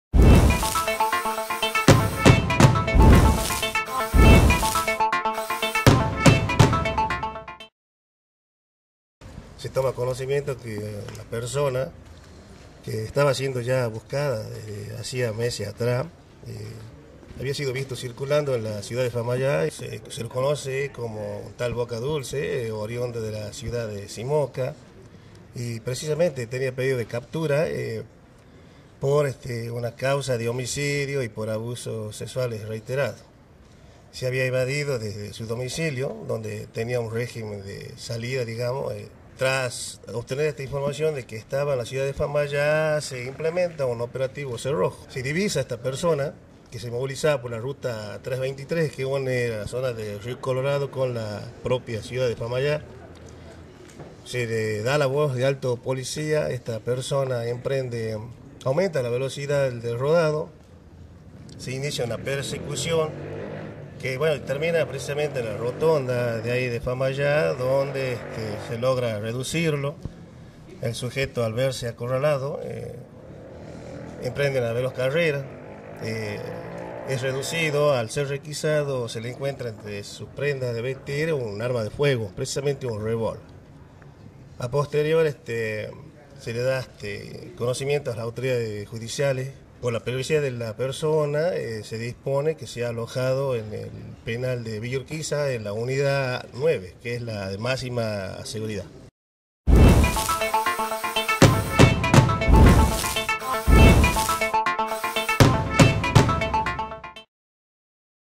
Arrestaron a un prófugo condenado por homicidio y violación
Personal policial de la Unidad de Investigaciones de la Unidad Regional Oeste, logró recapturar en Famaillá a un criminal conocido como “Boca Dulce” que estaba evadido de la justicia y era intensamente buscado. Cumplía condena por homicidio y reiteradas violaciones.

“Se había tomado conocimiento que una persona que era buscada desde hacía meses fue visto en Famaillá a bordo de una motocicleta de gran cilindrada color negro, ocultando su identidad con un casco azul”, explicó el jefe del área, Pedro Gómez, quien además destacó que “Boca dulce” fue recapturado luego de implementar un operativo cerrojo.
“Se lo divisó cuando se movilizaba por ruta 323 en la zona de Río Colorado. Se inició una persecución donde el malviviente trató de huir y terminó su fuga en la rotonda de Famaillá donde fue detenido. El hombre fue requisado y se le secuestró un arma de fuego calibre 32”, declaró.

El sujeto había huido de su domicilio el 10 de mayo, incumpliendo el Régimen de Salidas que gozaba con permiso especial y se había quitado la pulsera electrónica. Tras ser detenido, quedó a disposición de las autoridades judiciales de Monteros, donde se dispuso que sea alojado en el Penal de Villa Urquiz